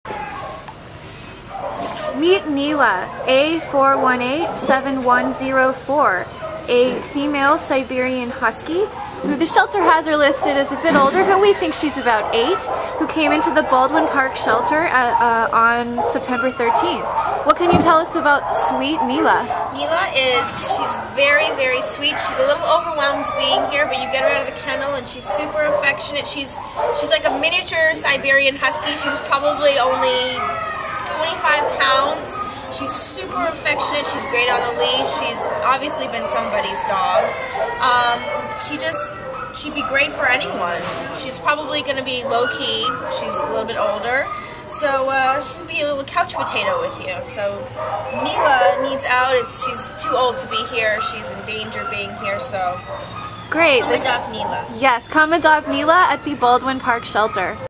Neela A4187104 ADOPTED!!!
Neela is a tranquil eight year old gray female Siberian Husky mix who was dumped at the Baldwin Park Shelter by her former owner on September 13th for absolutely no reason that we know of.  Neela weighs twenty five pounds, and was described by our volunteer as a "pygmy husky".  She is strikingly beautiful (she has one blue and one brown eye), is good with other dogs and walks well on leash.  Neela has a low energy level and is happy to be your couch potato pal.  She will be an excellent indoor pet for any living situation.